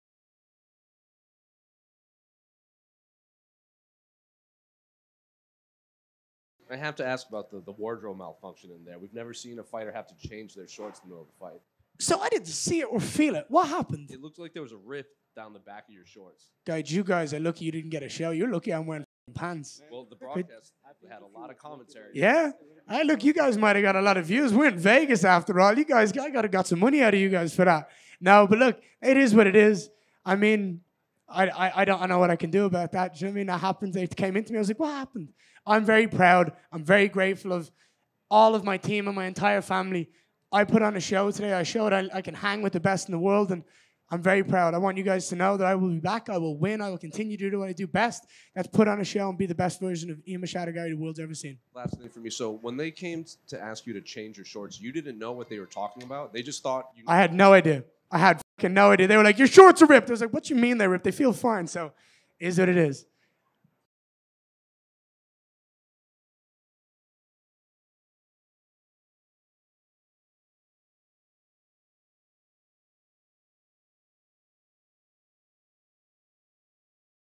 Ian Machado Garry Reacts to RIPPING his shorts "Your Lucky I'm wearing pants !"
Ian Machado Garry reacts to the moment he was asked to change his shorts due to tearing them during his fight against Shavkat Rakhmonov at UFC 310